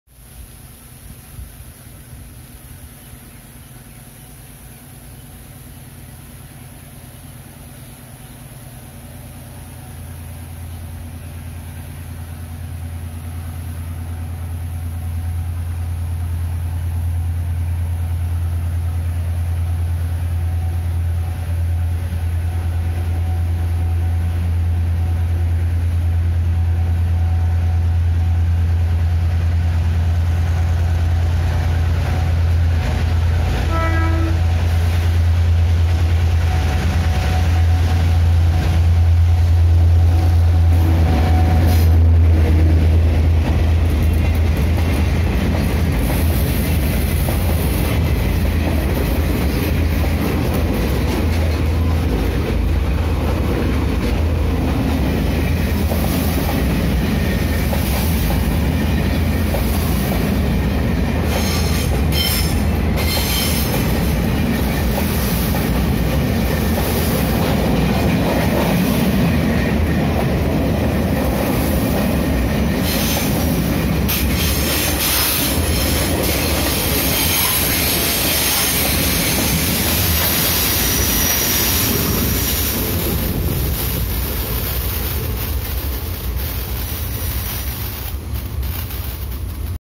台鐵 柴電機車R169牽引 7501次 散裝貨列 通過三貂嶺/Taiwan EMD G22-CU R169 leading freight cars pass through Sandiaoling
台鐵 柴電機車R169牽引 7501次 散裝貨列 通過三貂嶺

Taiwan EMD G22-CU R169 leading mix freight cars pass through Sandiaoling

時間/time: 10:49 a.m.